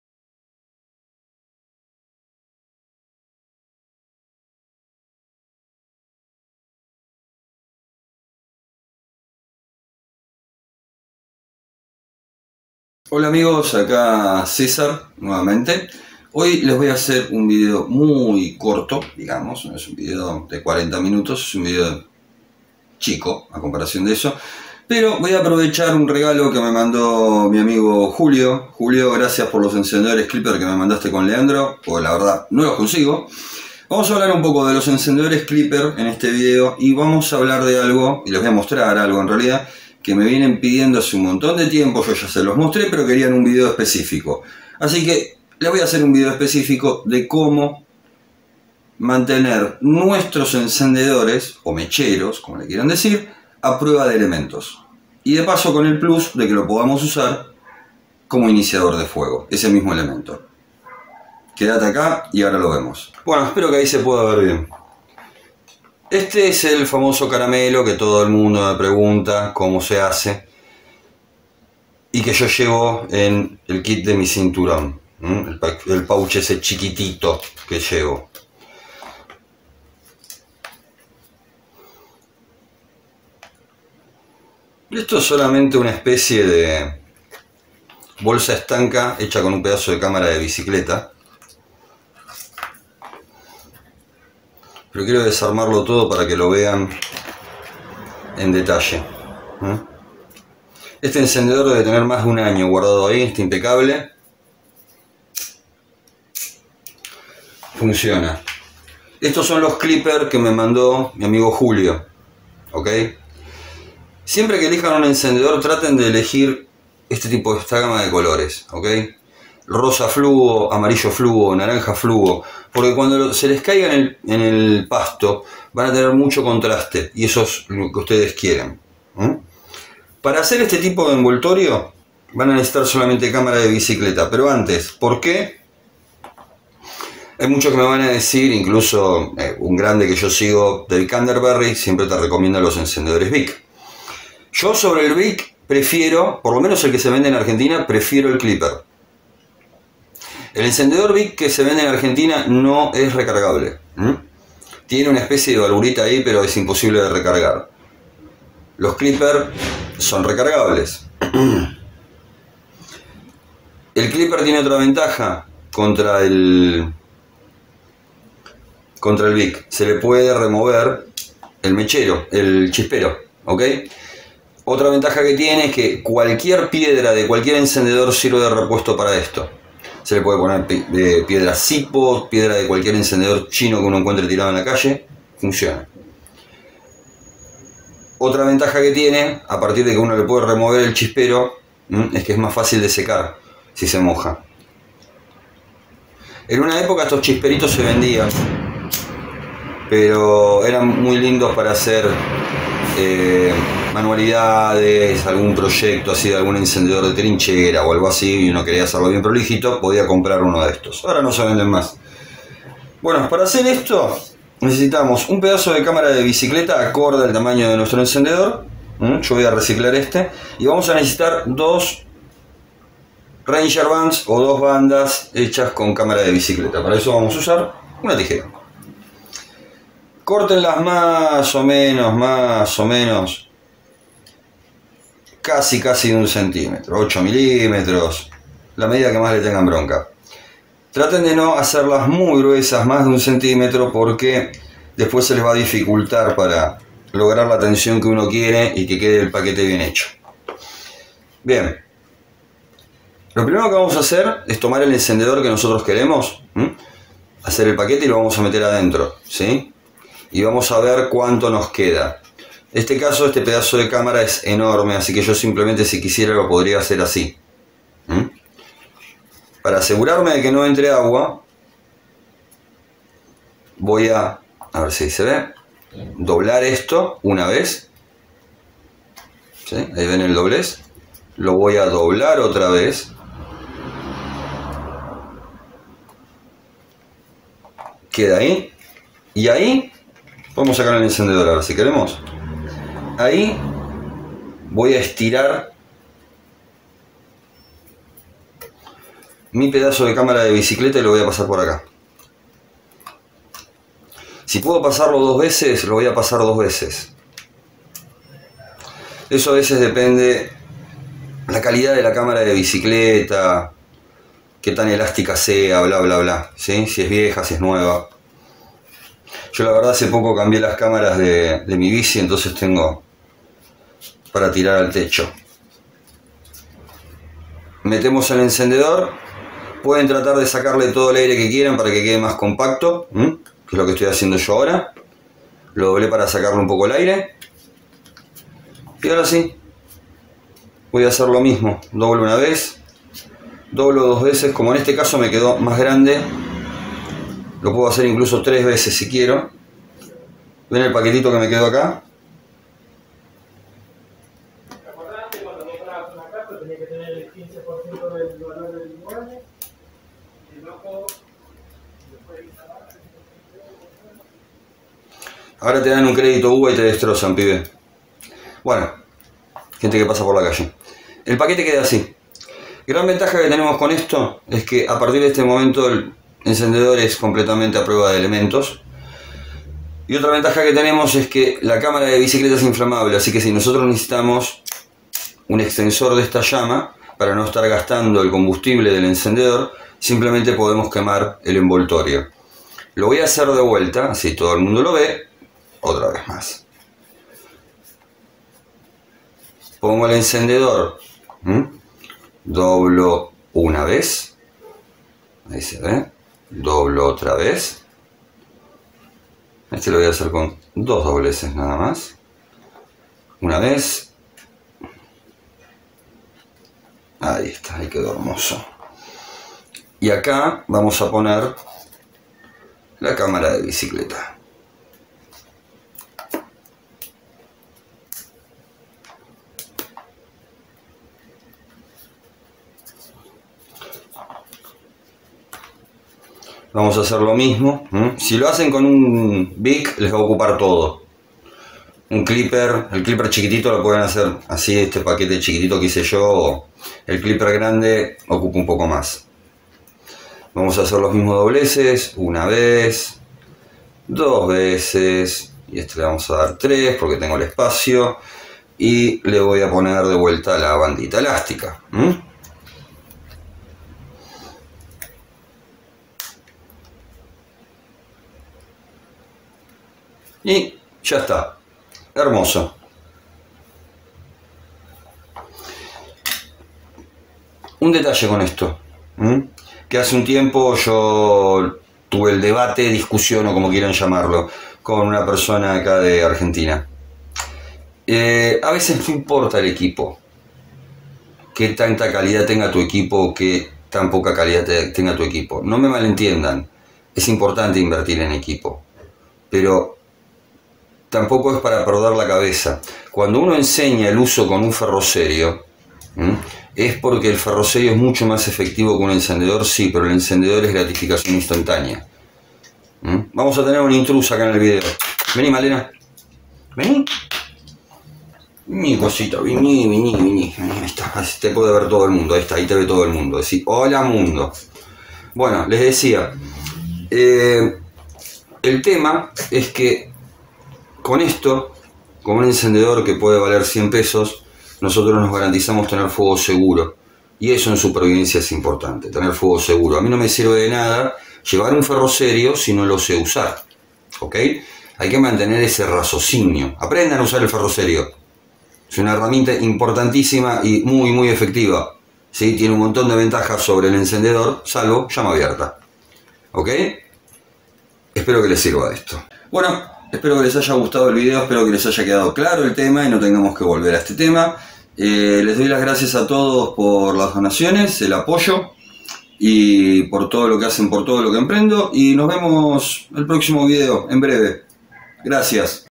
Volve tu encendedor/mechero a prueba de agua
En esta oportunidad y a respuesta de muchos pedidos, les muestro como volver tu encendedor/mechero a prueba de agua con un material que funciona como extensor de llama y tambien comparamos el encededor Bic vs Clipper.